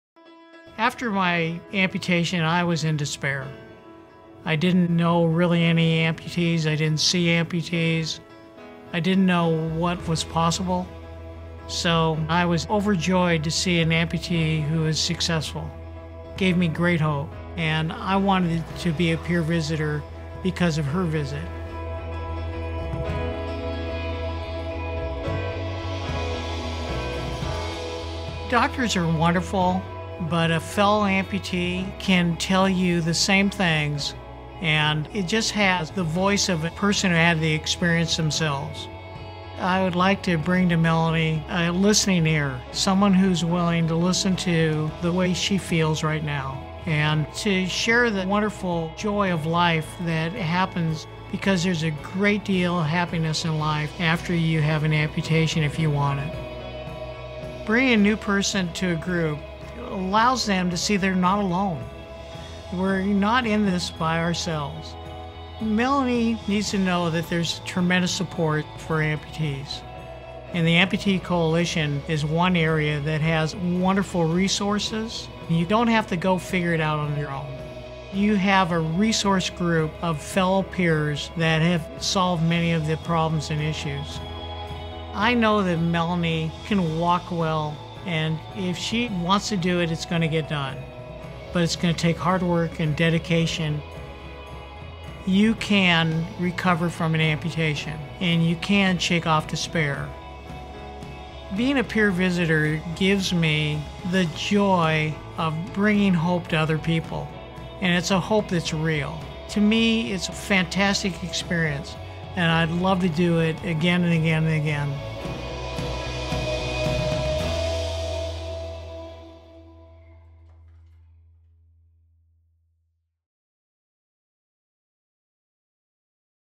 Amputee Rehabilitation Series: Video #3 – A Community of Peers
Talking with and being around other individuals with limb loss helps to normalize your experience and reassure you that a full life is possible after the loss of a limb. Fred Samuelson, an Amputee Coalition-certified peer visitor, reflects on how a peer visit impacted him after the loss of his leg and the role that certified peer visitors play in the recovery process. Receiving a peer visit helped Fred find hope after the amputation of his right leg below the knee due to complications from vascular disease.